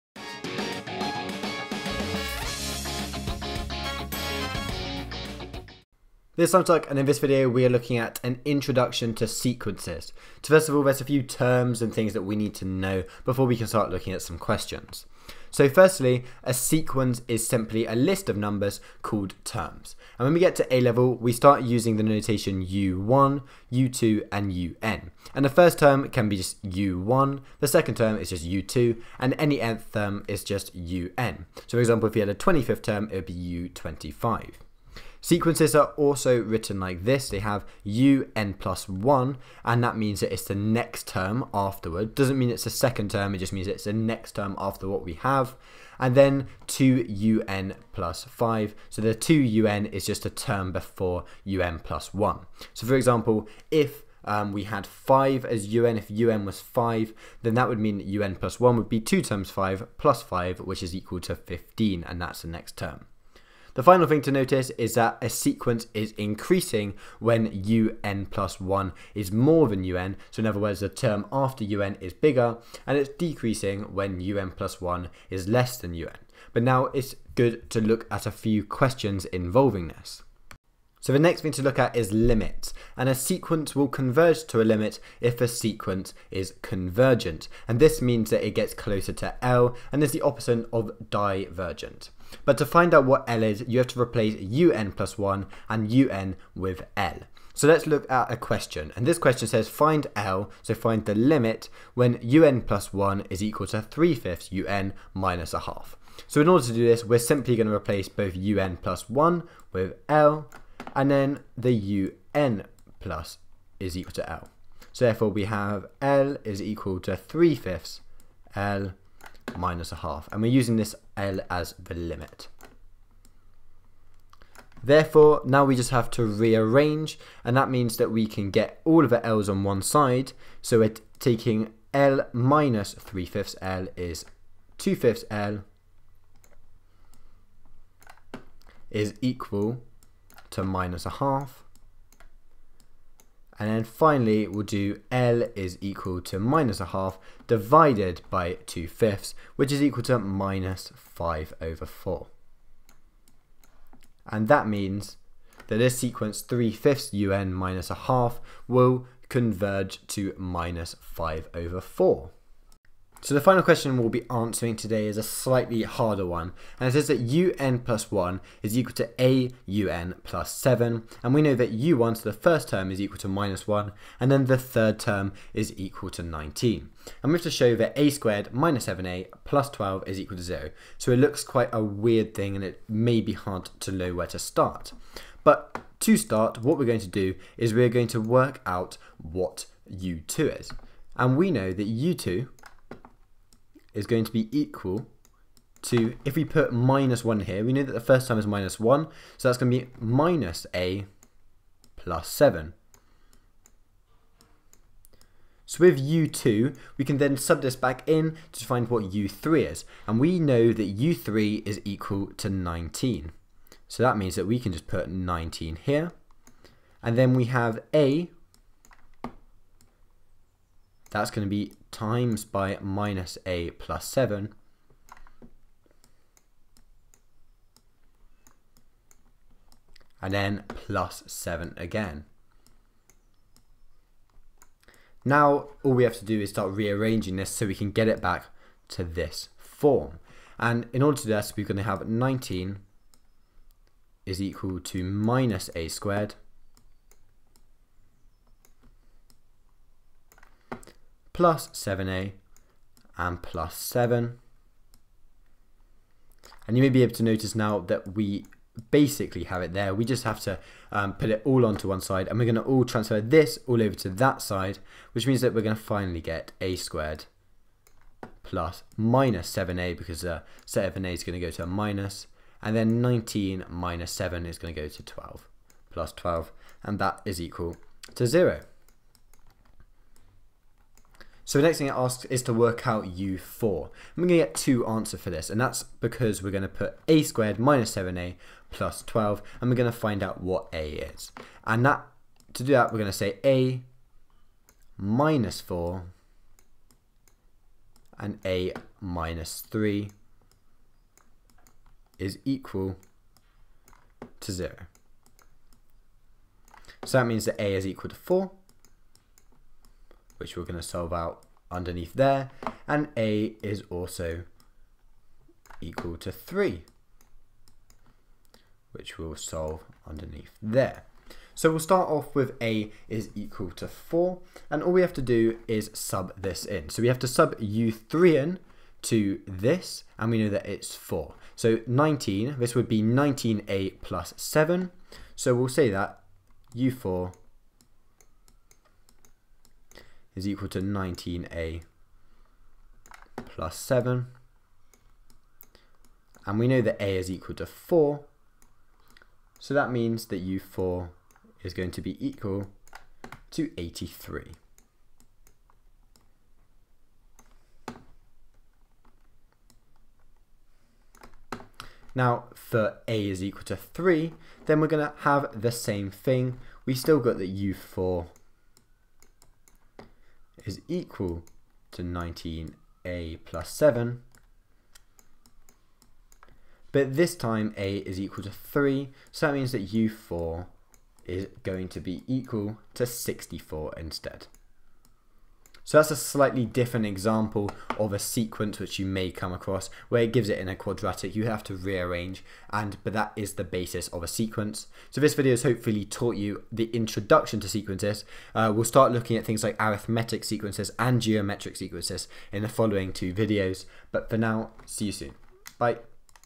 Introduction to Sequences - A-Level Maths
This video looks at an introduction to A-Level sequences. This includes convergent and divergent sequences, as well as a look on limits.